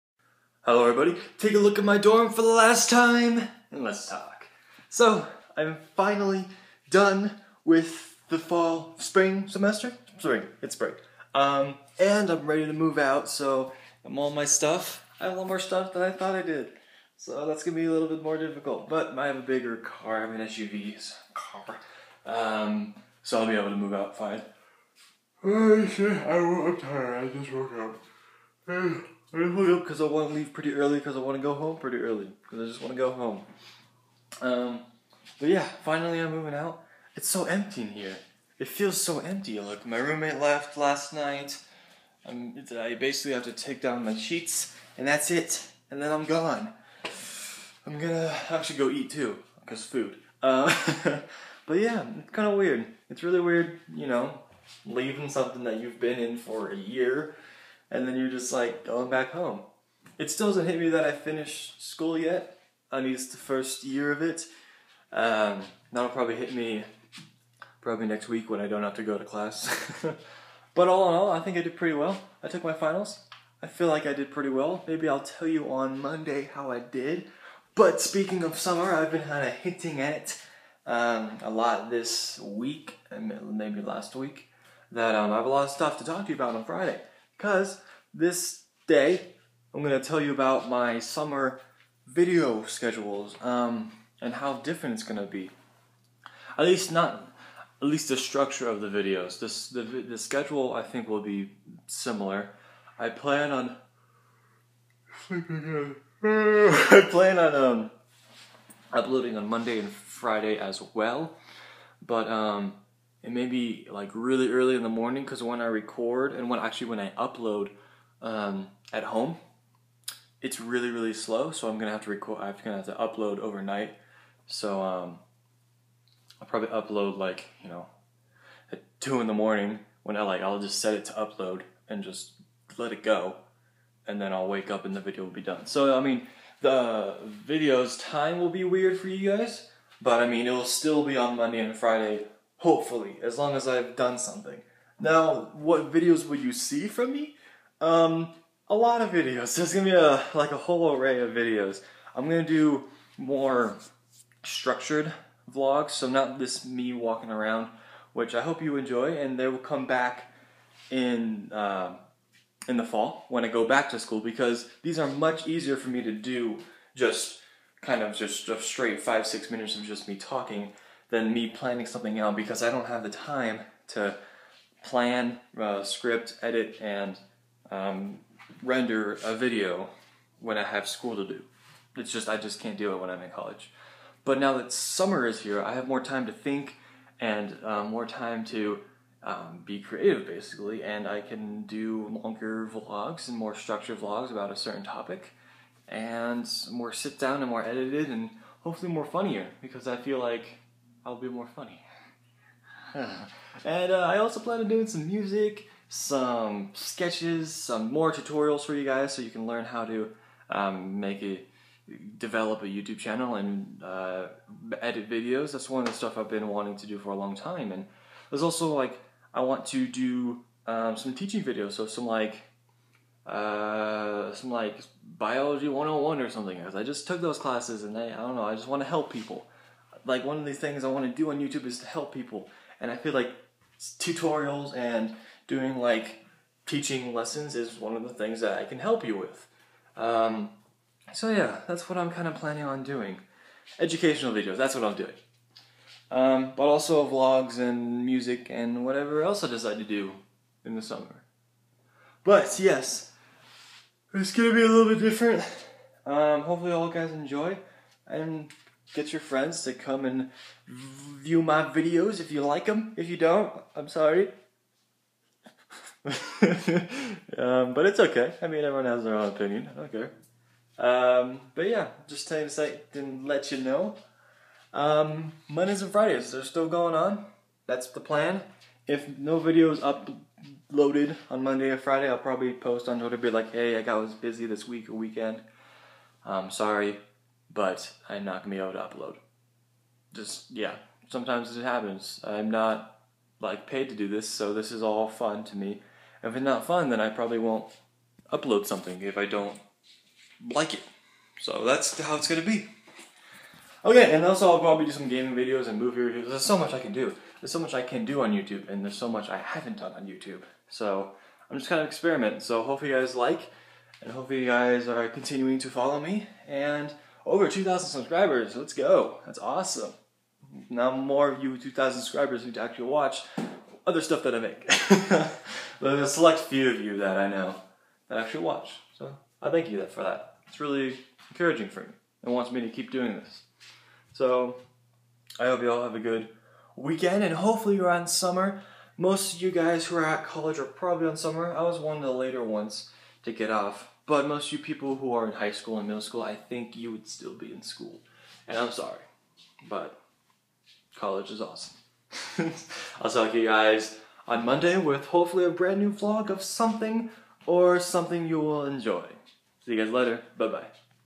School is over, time for summer!!!
School is finally over and now i get to relax. Today i tell you my plans this summer for YouTube and what videos i'm going to make.